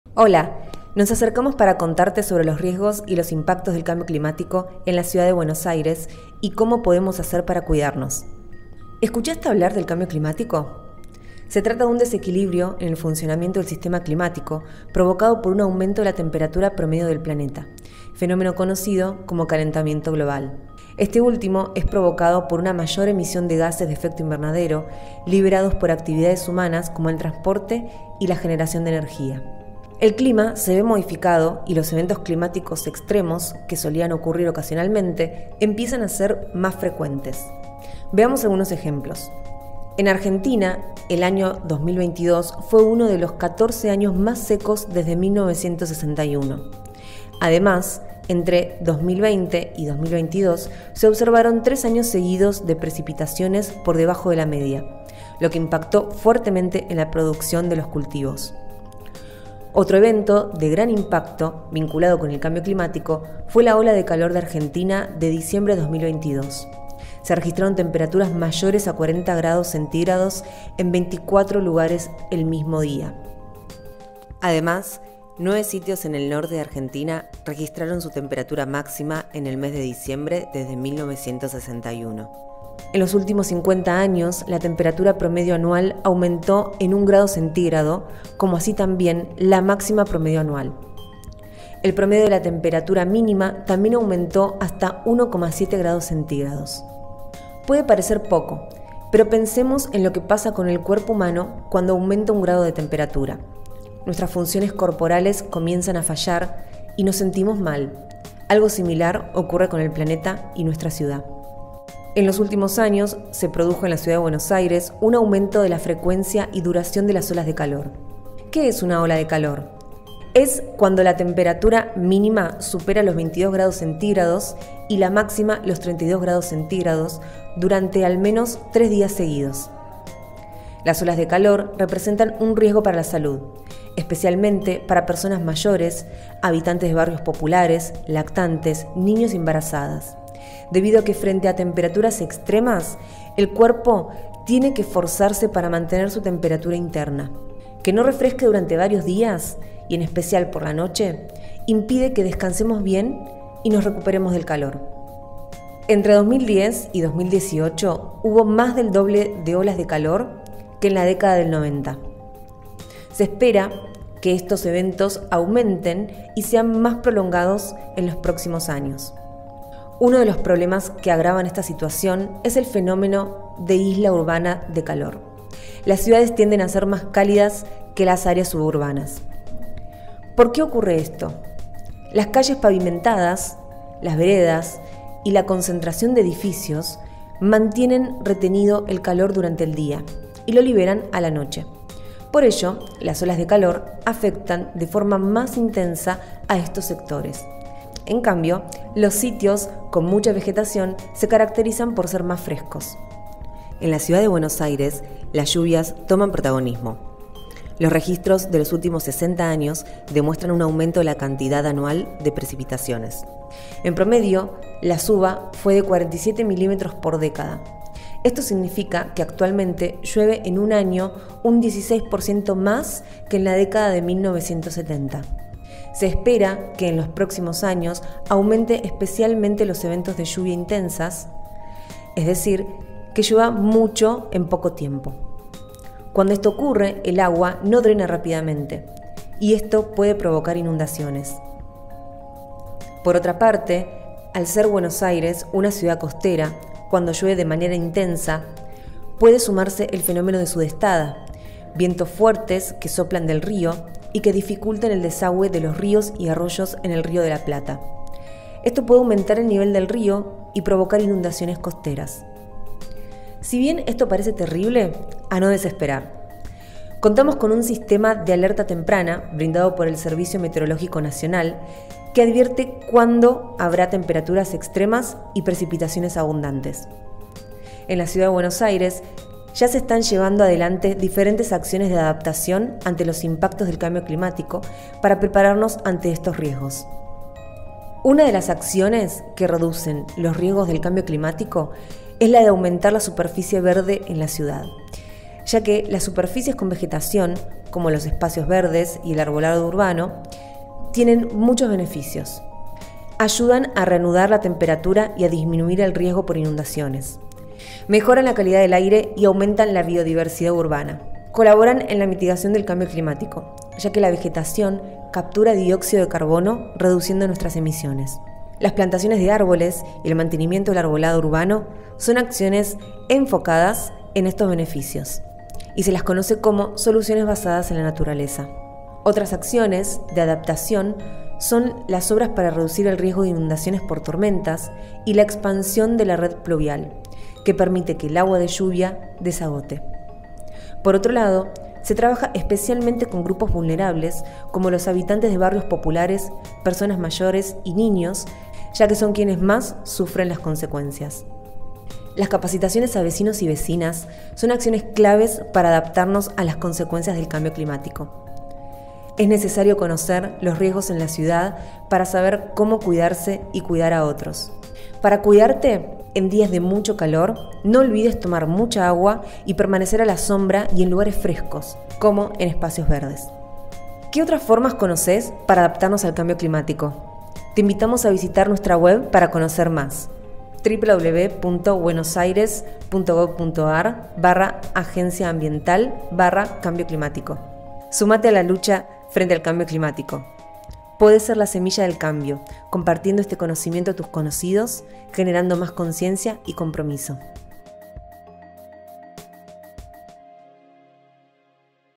Riesgos climáticos y estrategias de adaptación para la Ciudad de Buenos Aires
Este video explica los riesgos climáticos de la Ciudad de Buenos Aires y cuáles son las estrategias de adaptación que se llevan a cabo para reducir los riesgos frente a las principales amenazas climáticas.